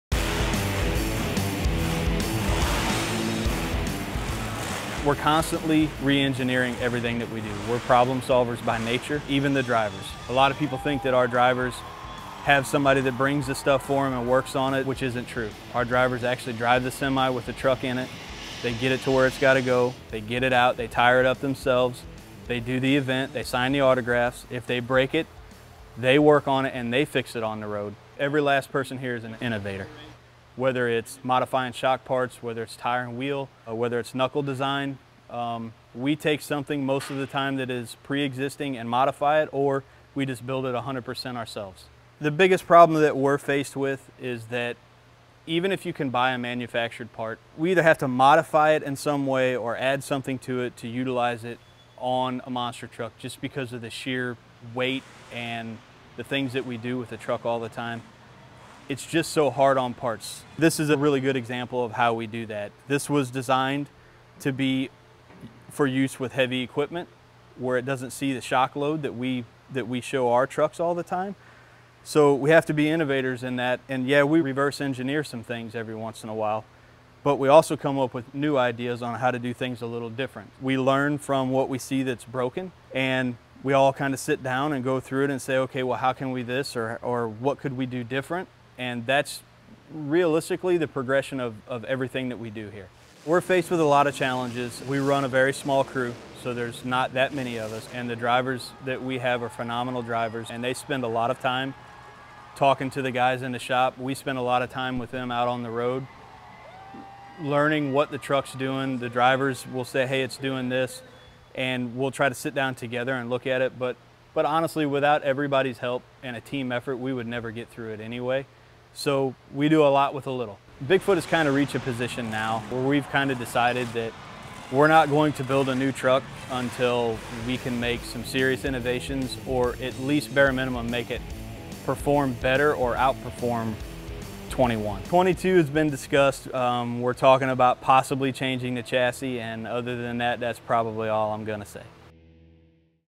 BIGFOOT 4x4 Technology: BIGFOOT 4x4 is a Giant Among Innovators in the Monster Truck Industry
BIGFOOT 4x4's Bryan Bertoletti spends long hours in the shop and on the road for shows. He's the shop foreman and BIGFOOT 4x4's leader of research and development. Bertoletti describes his crew and drivers as innovators! Take a look inside BIGFOOT 4x4's shop in Pacific, Missouri. This segment kicks off a series of videos highlighting the innovative automotive technology involved with the company's latest truck, BIGFOOT 21.

Kathleen Berger
Executive Producer, HEC Science & Technology